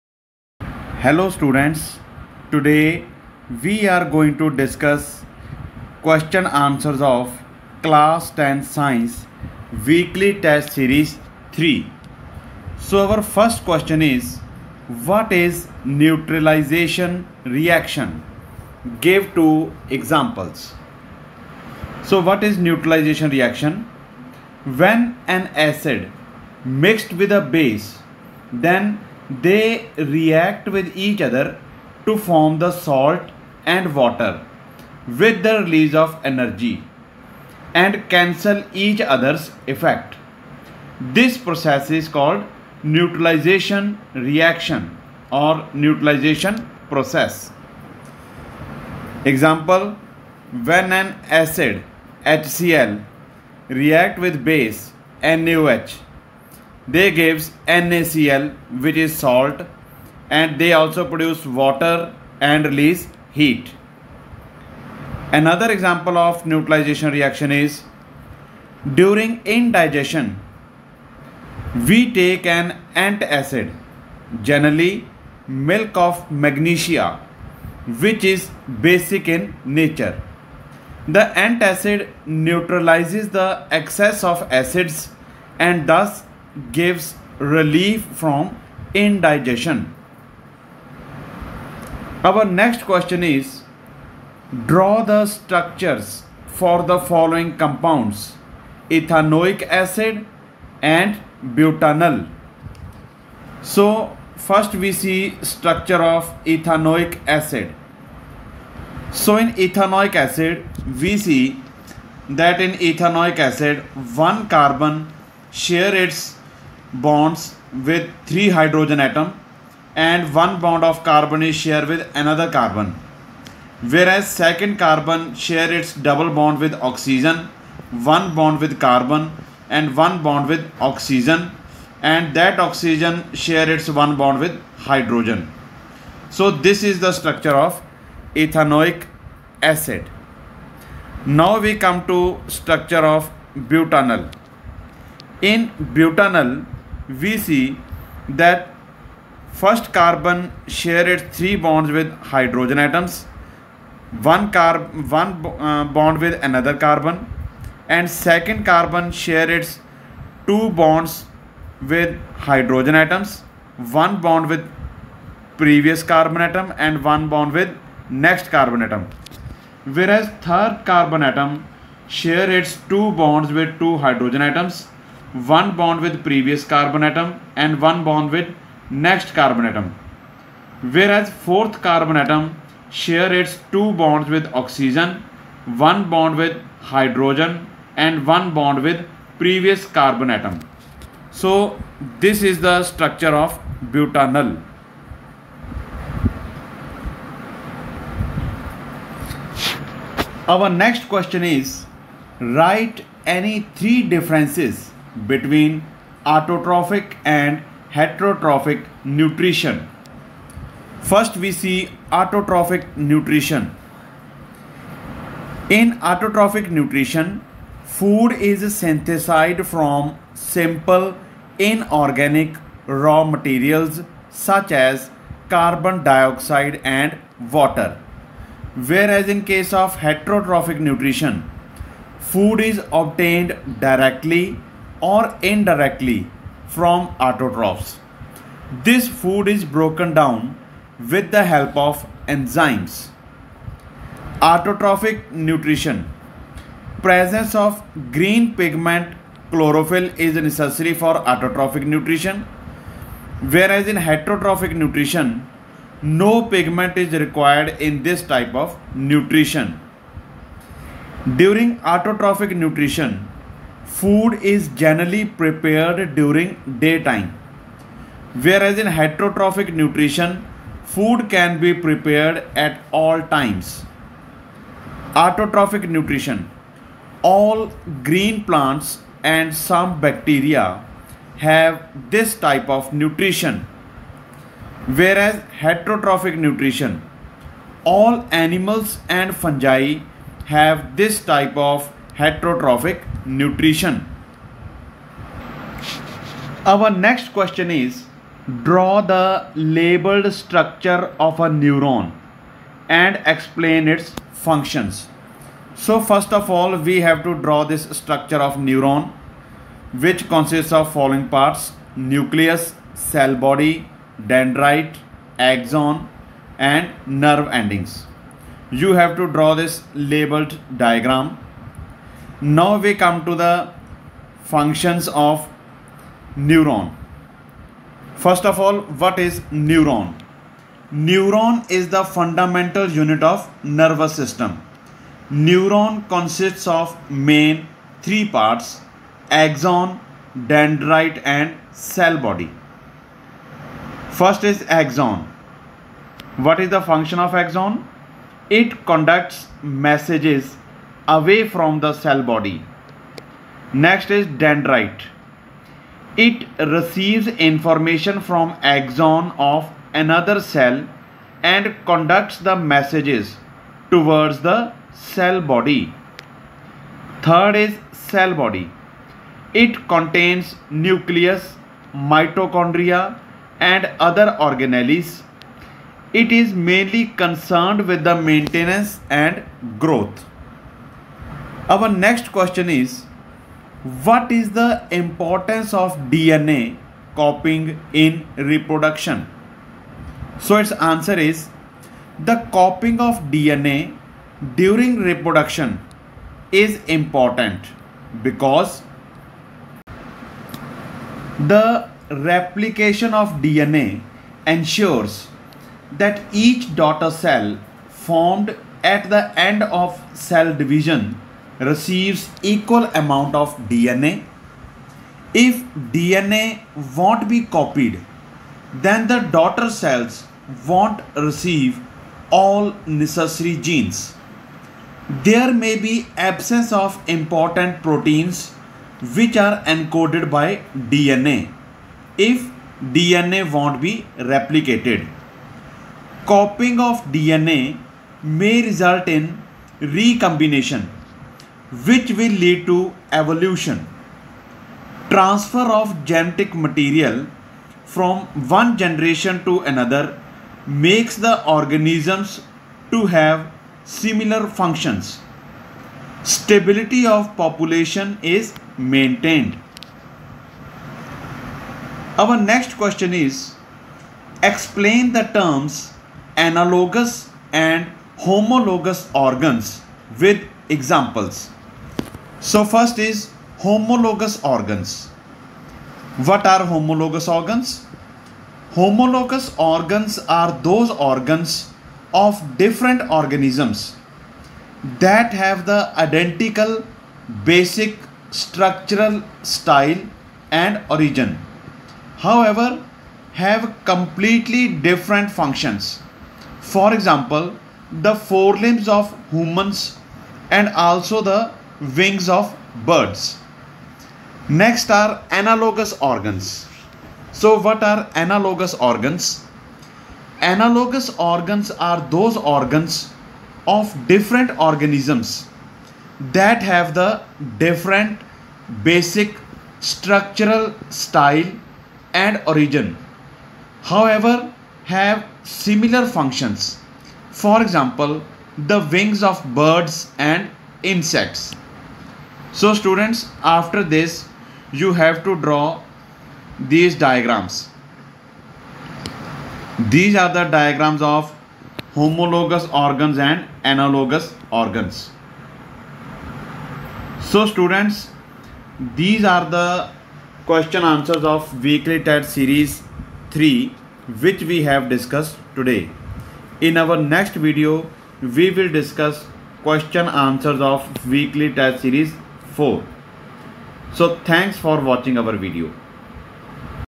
CLASS10TH SCIENCE FULLY SOLVED WEEKLY TEST SERIES 3 ( ENGLISH MEDIUM ).
In this video we have discussed question answers of  Class10th Science  weekly test series 3  in  English Medium .
@Dhanjal tutorials Science